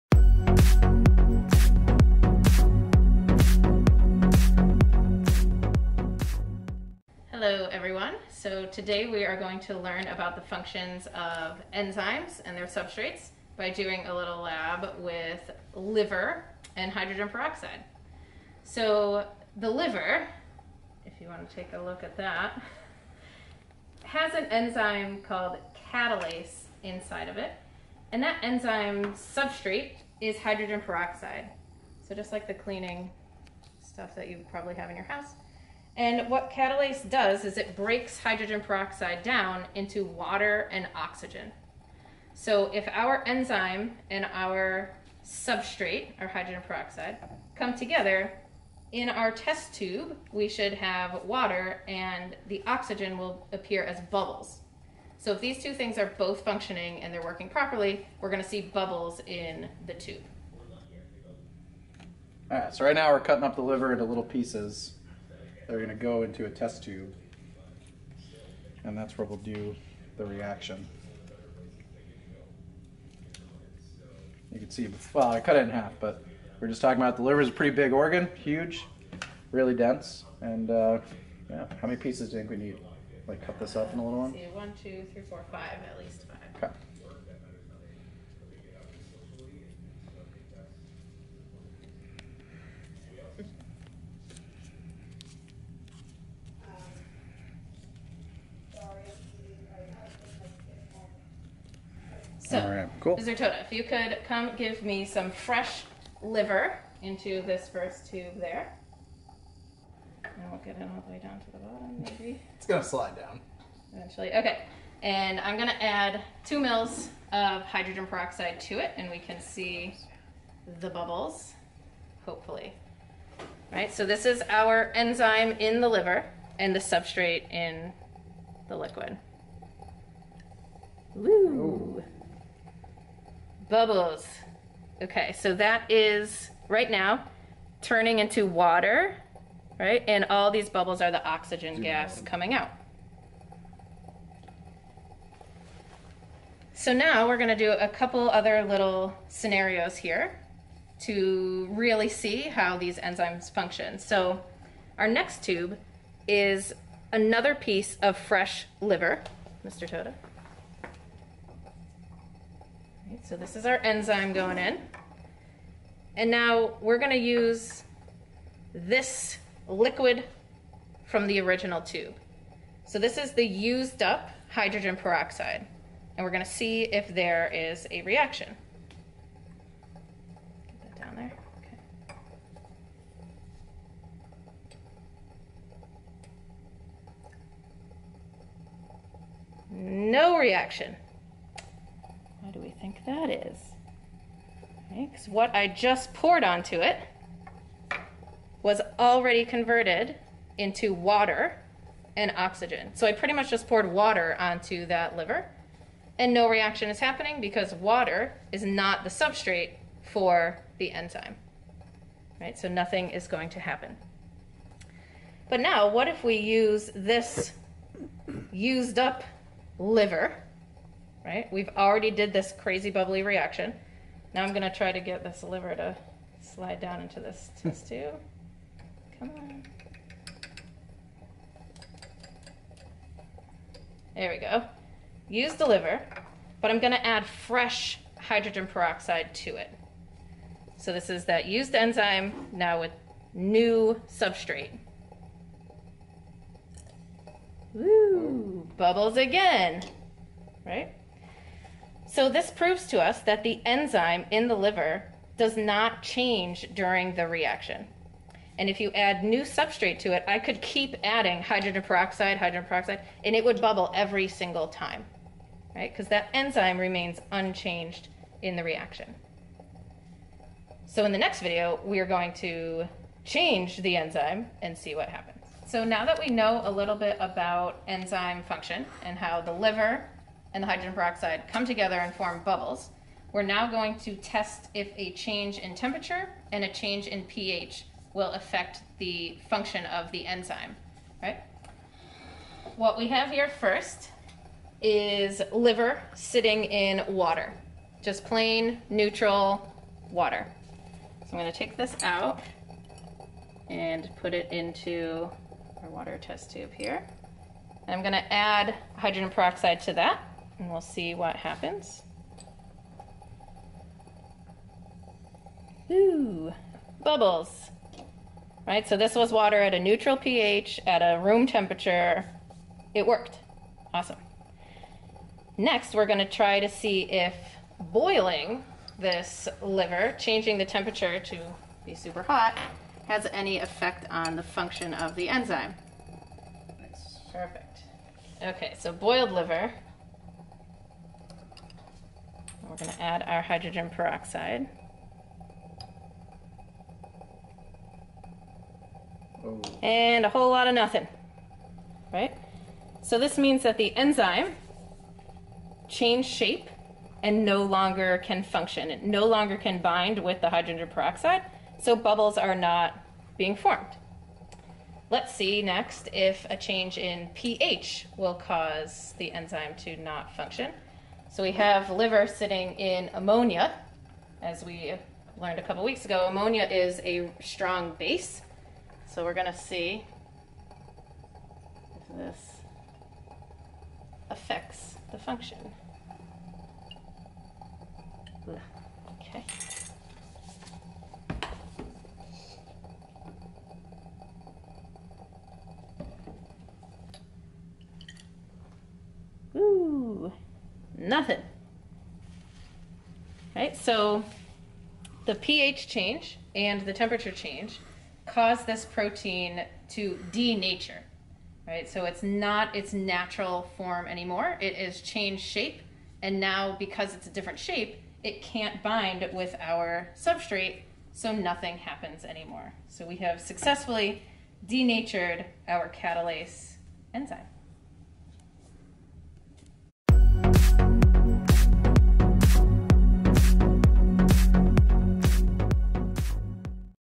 Investigating Enzymes in Liver
This video explores how the enzyme "Catalase" in the liver breaks down Hydrogen Peroxide. This video demonstrates what happens when you "denature" or change the shape of enzymes with temperature of pH.